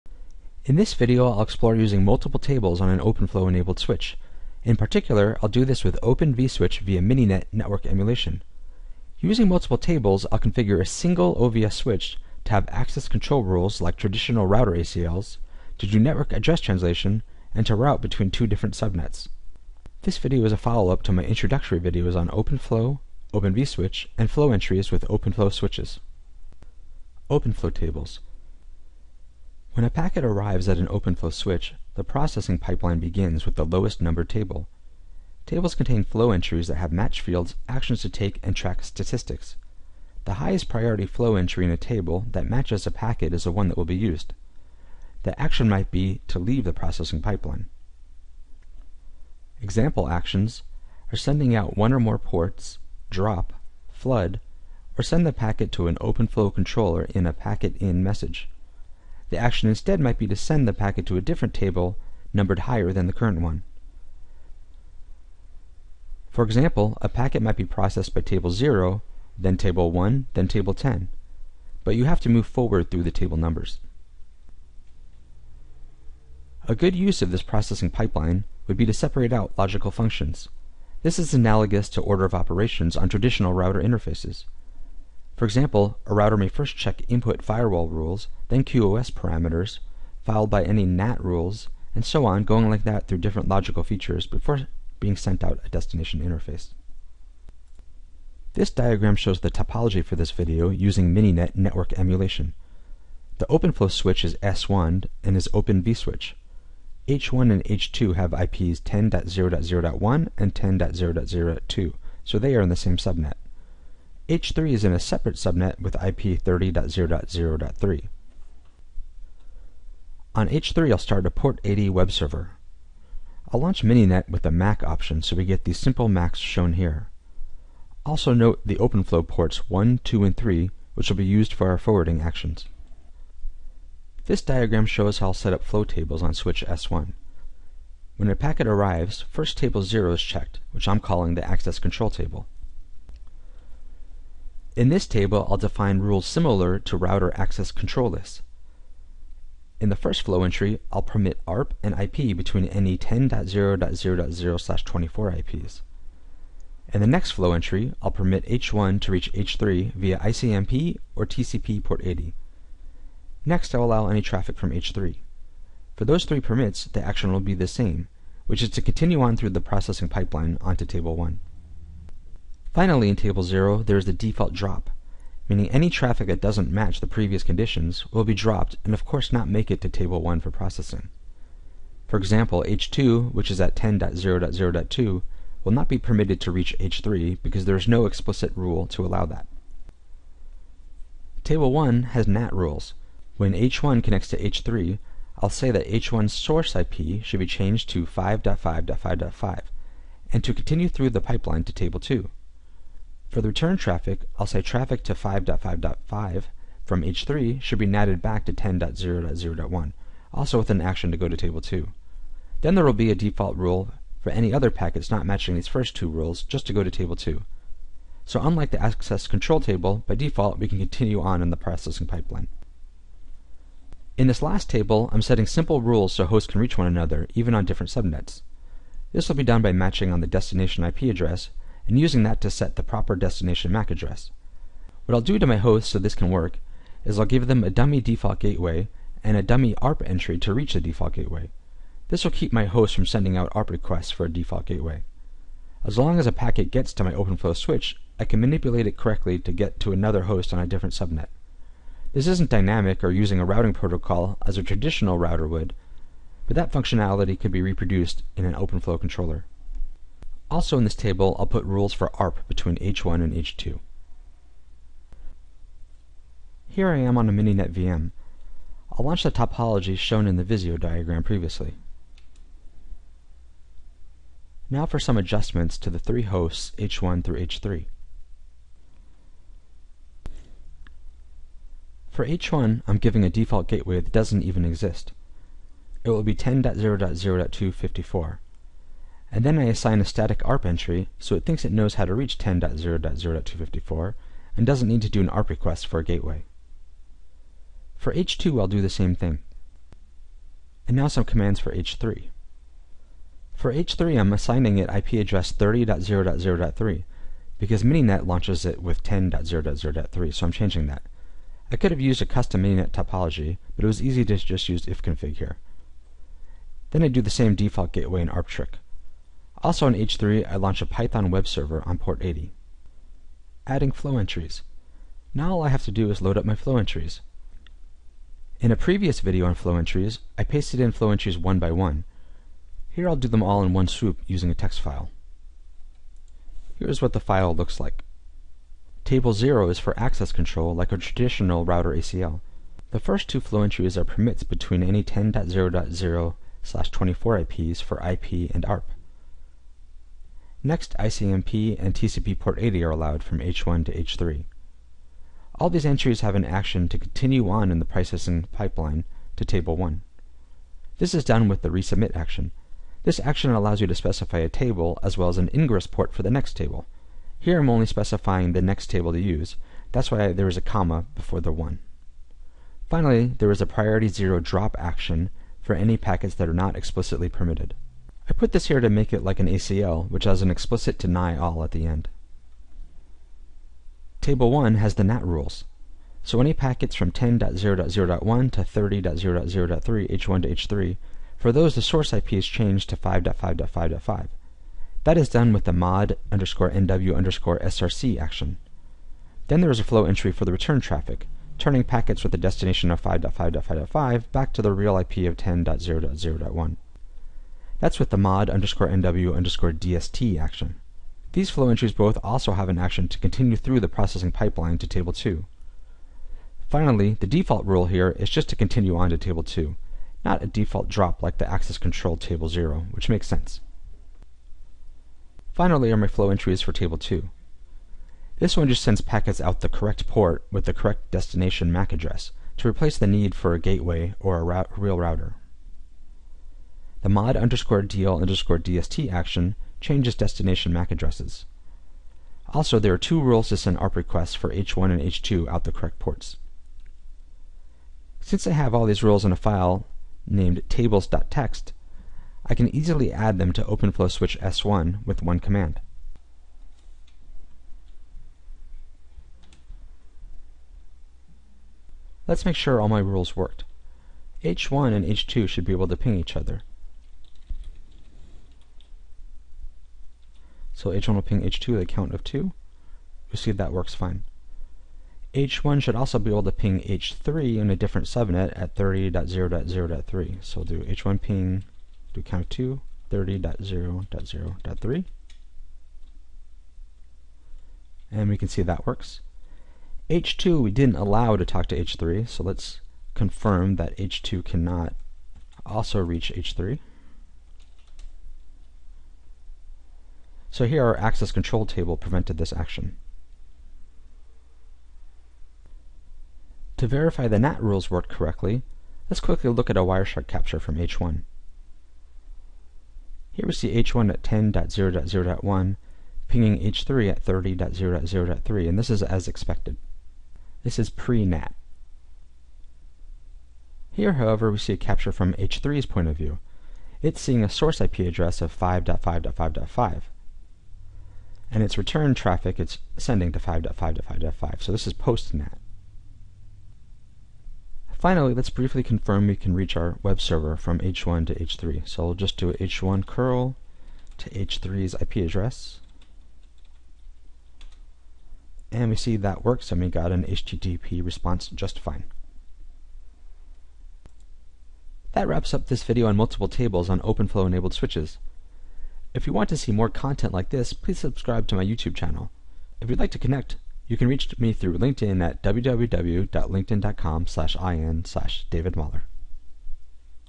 OpenFlow with multiple Flow Tables
This video looks at using multiple flow tables on a single OpenFlow enabled switch.  I create a table used for access control, a table for NAT and a table for routing.  This demo does this on Open vSwitch  (OVS) and with Mininet Network Emulation.